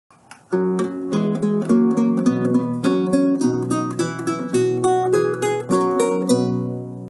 P60-2
フランスリュート協会教則本　Le secret des Musesの
60ページ下の練習曲2です

intabulated by Pascale Boquet

played by Tomoko Koide